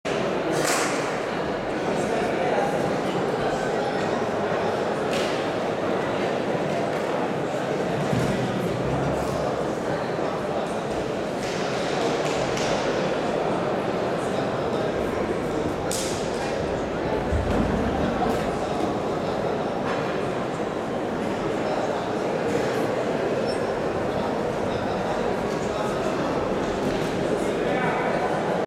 W Long Jump 25
U18 Romanian National Indoor Championship - Day 2 - 2 March 2014 - Bucharest